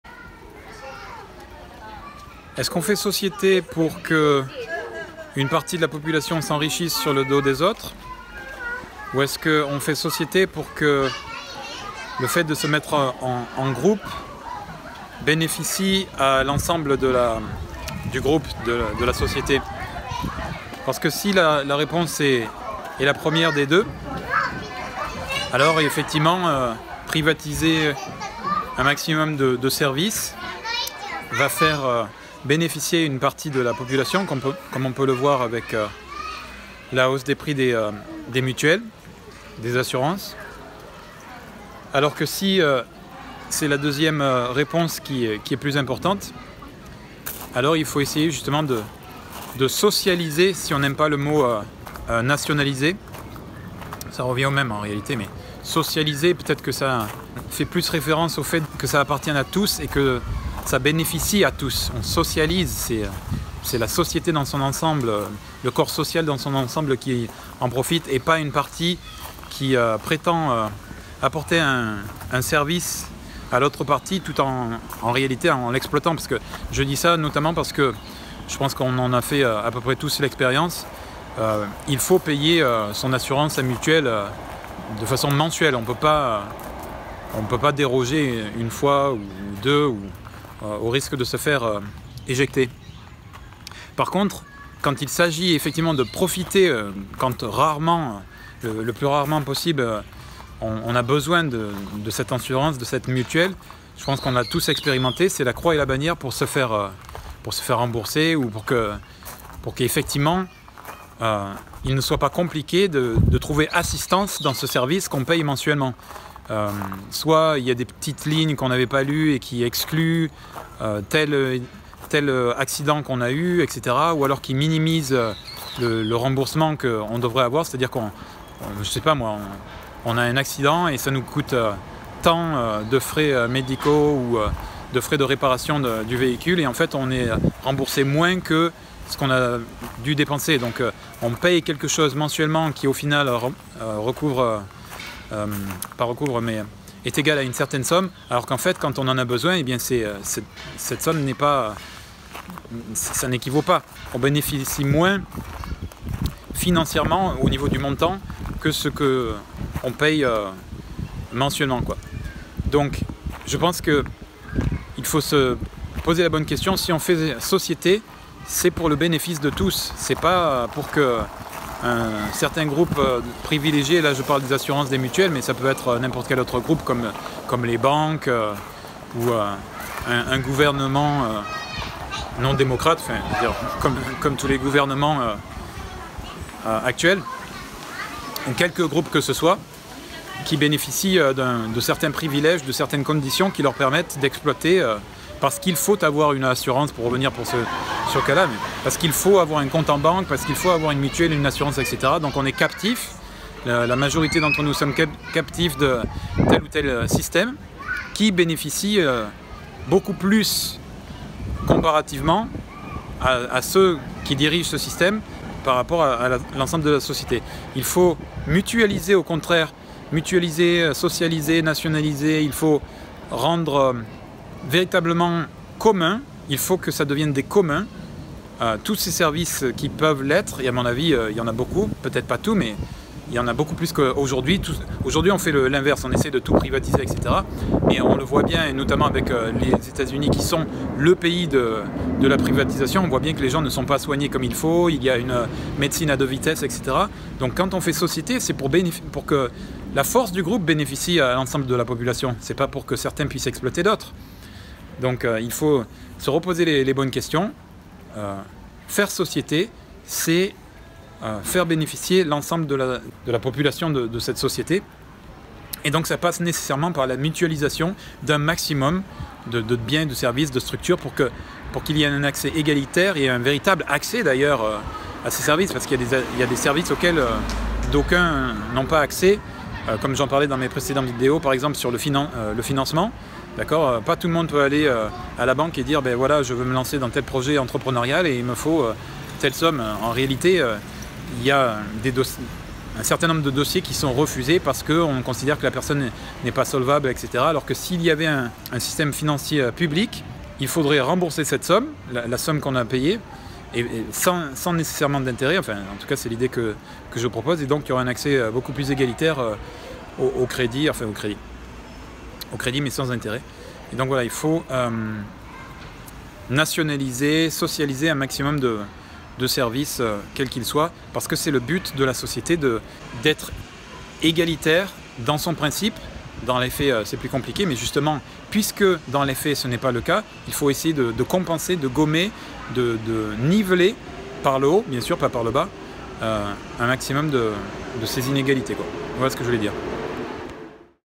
L'Échec de la Privatisation
A chaque privatisation est observer son lot d'augmentation drastique des prix, des conditions d'accès et pourtant de dégradation de qualité de service.

Il faut le constater, la privatisation est un échec. Contrairement a ce qui est affirmé le secteur privé, à cause de sa logique capitaliste, gère moins bien que la collectivité.

Une humanité mature dans une société juste, socialise un maximum de biens et services pour en garantir l'accès équitable au plus grand nombre.